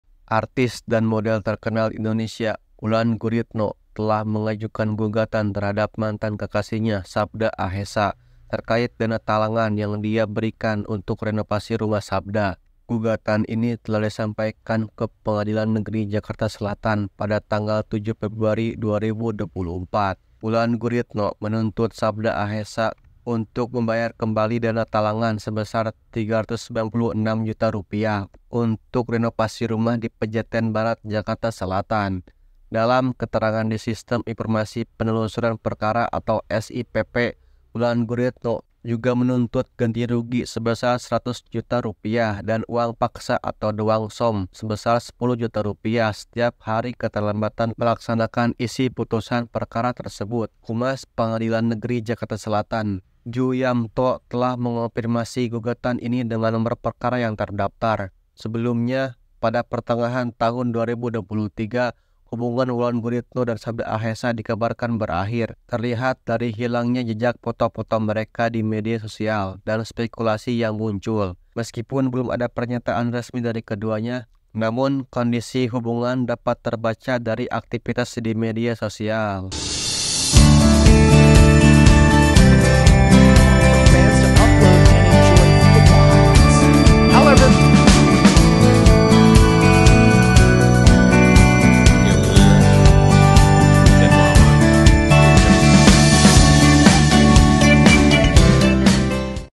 Wulan Guritno Gugat Mantan Pacar Rp400 Juta
Artis dan model terkenal Indonesia, Wulan Guritno, telah mengajukan gugatan terhadap mantan kekasihnya, Sabda Ahessa, terkait dana talangan yang dia berikan untuk renovasi rumah Sabda. Gugatan ini telah disampaikan ke Pengadilan Negeri Jakarta Selatan pada tanggal 7 Februari 2024, dengan nomor perkara 5/Pdt.G.S/2024/PN JKT.SEL. Wulan Guritno menuntut Sabda Ahessa untuk membayar kembali dana talangan sebesar 396 juta rupiah untuk renovasi rumah di Pejaten Barat, Jakarta Selatan.

Dalam keterangan di Sistem Informasi Penelusuran Perkara (SIPP), Wulan Guritno juga menuntut ganti rugi sebesar 100 juta rupiah dan uang paksa (dwangsom) sebesar 10 juta rupiah setiap hari keterlambatan melaksanakan isi putusan perkara tersebut. Humas PN Jakarta Selatan, Djuyamto, telah mengonfirmasi gugatan ini dengan nomor perkara yang terdaftar.

Support :
Merasa puas dengan video ini,? boleh dong dukung perkembangan Channel Potensi Bisnis disini